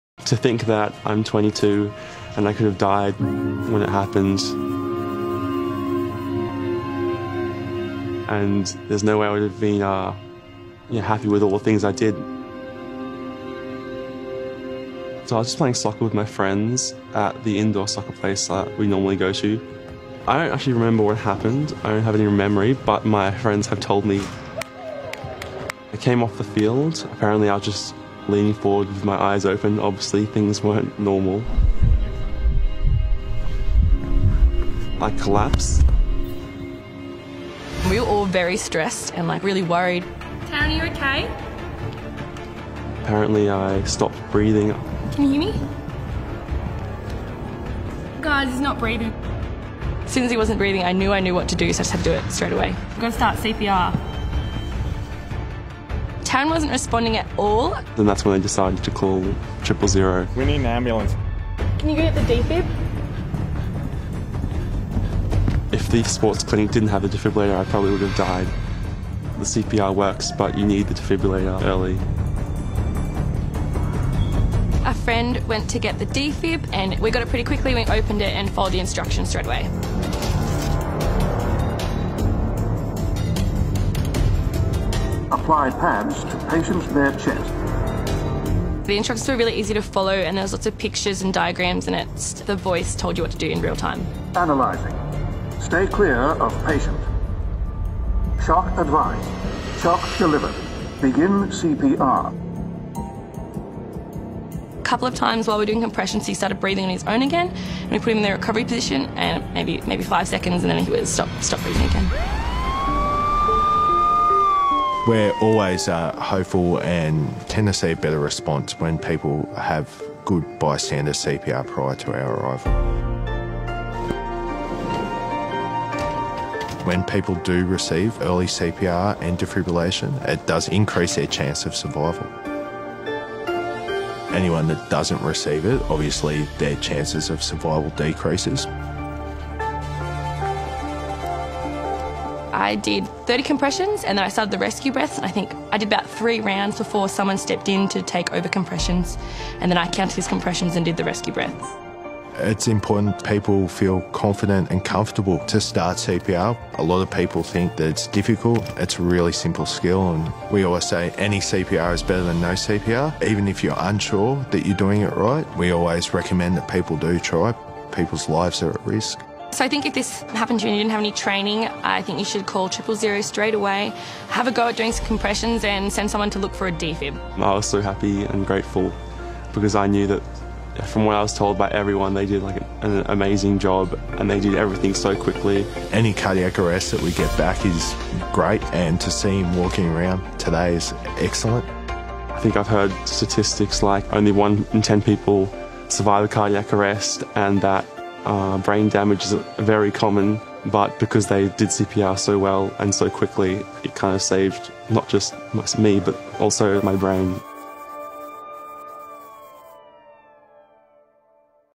Restart A Heart Day - Tarran's Story
Anyone can have a cardiac arrest, and anyone can restart a heart.
Tarran’s quick-thinking friends saved his life by following the 3 simple steps.
Call. Push. Shock.
Refresh your skills this Restart a Heart Day (16 October).